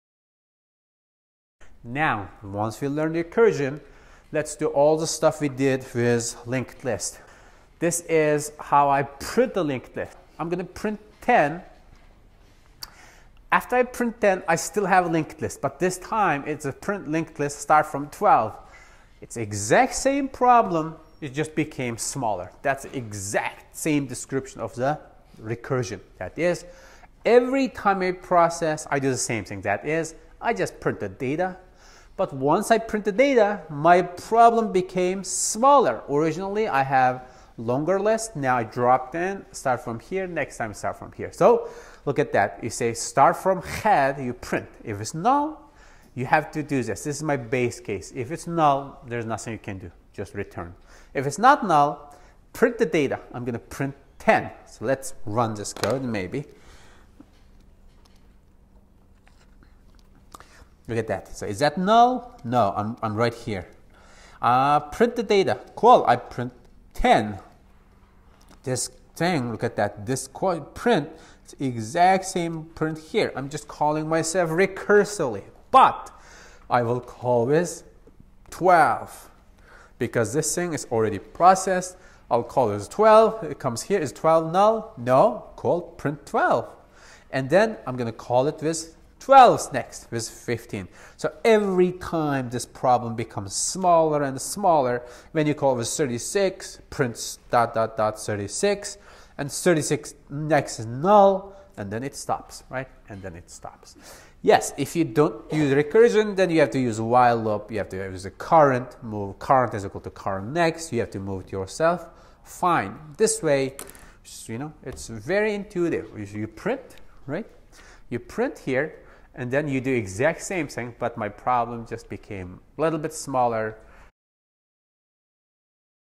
Recursively print a linked list
This video shows how to print a linked recursively in Java